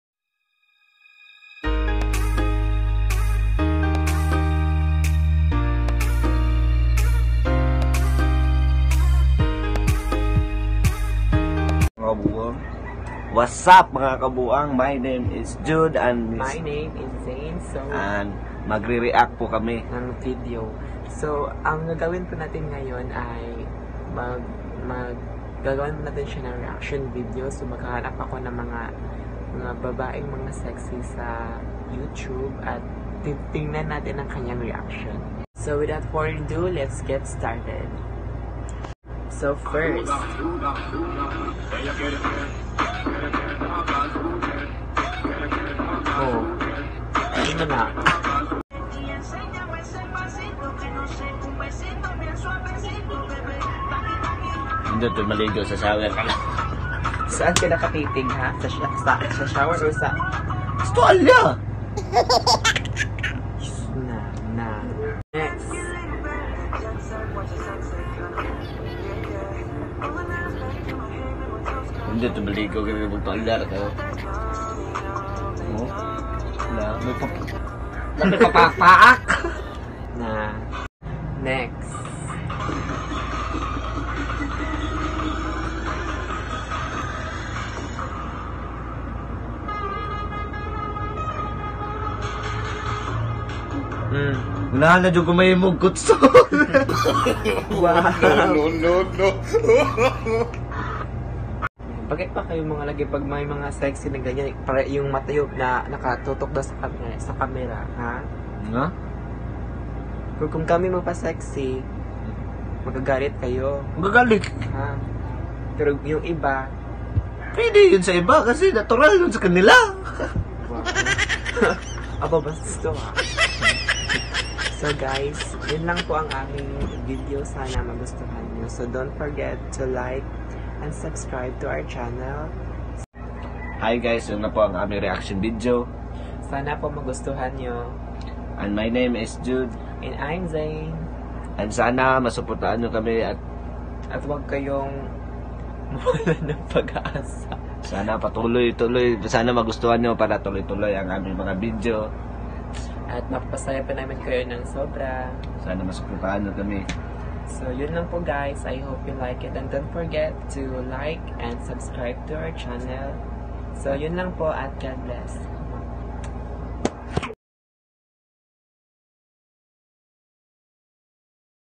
MY FIRST REACTION VIDEO VLOG! TOGETHER WITH MY BOYFRIEND❤
Hi guys, komusta na po kayo? I know its been a while hindi ako nakapag upload ng new video, pasensya na kayo guys medyo busy lang. 😊Today my vlog is all about reaction.And  this is my first reaction video vlog,! together with my boyfriend.❤ Actually our first vlog together guys! yay!😍 so happy and excited to do this video with him.! Please support our vlog coming out soon guys!👍 Mga tropa natin dyan! mga kabuang tandem.! lol!😂 I hope you guys enjoy this video.

Stay safe everyone! God bless!🙏

xoxo💋
Just Zyn